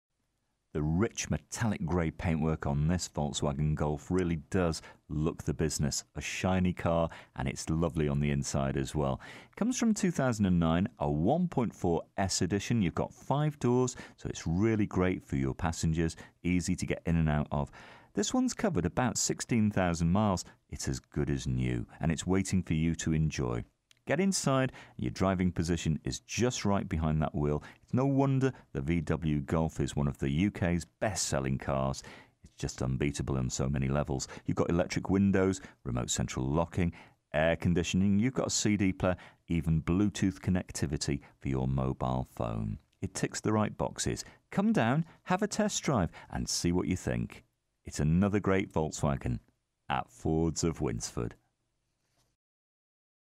USED VOLKSWAGEN GOLF HATCHBACK (2009) 1.4 S 5DR - AV58WWU
VOLKSWAGEN GOLF HATCHBACK (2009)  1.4 S 5DR

DETAILS:
First registered: 13/02/2009
Current mileage: 16,010
Registration: AV58WWU
Colour: GREY

FEATURES:
front electric windows, remote central locking, cloth interior, all round airbags, air conditioning, pas, abs, steel wheels, metallic paint, cd, traction control, parcel shelf present, hand book present, bluetooth